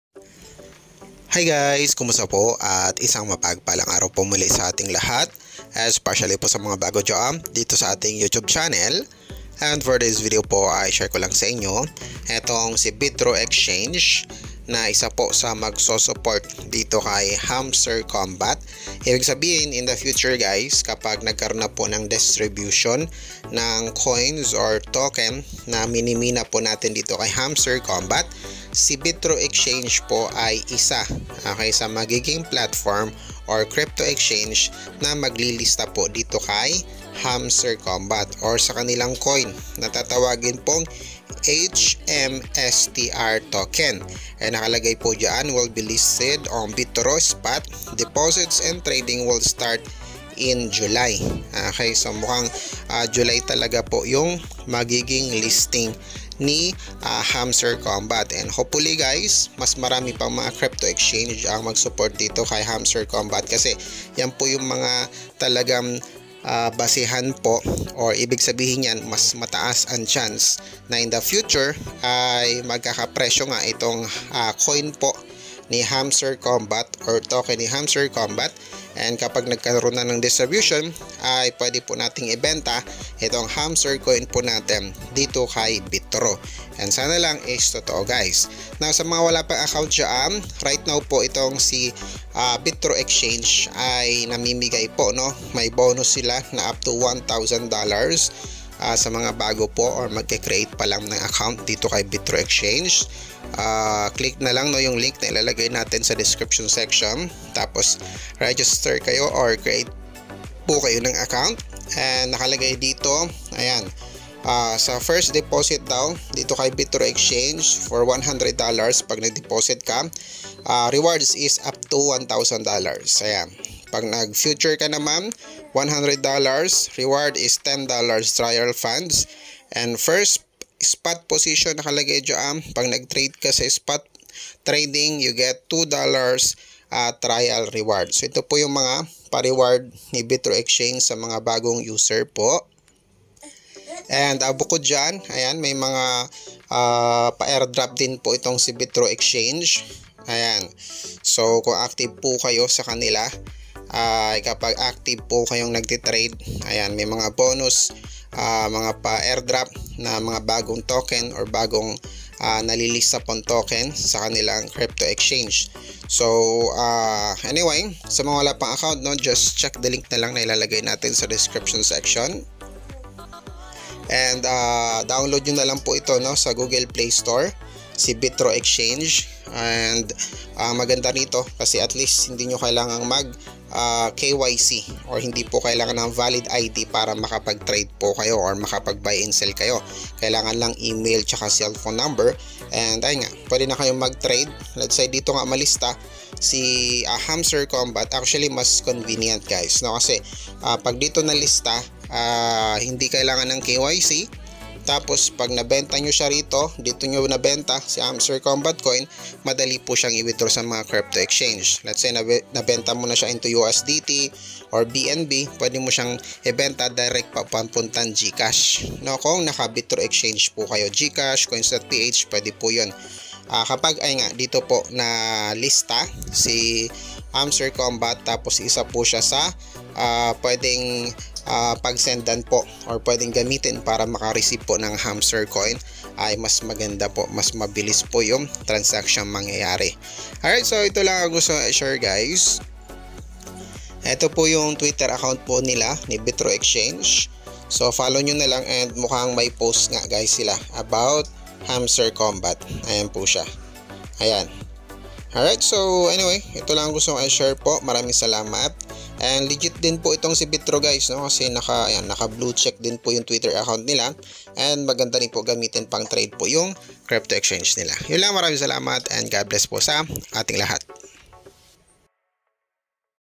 Hamster kombat supported ni Bitrue exchange and up to 1000$ ang reward😱
🐹 Hamster Kombat is taking Telegram by storm!

⚡️ $HMSTR will be listed on Bitrue spot
⚡️ Deposits and trading will start in July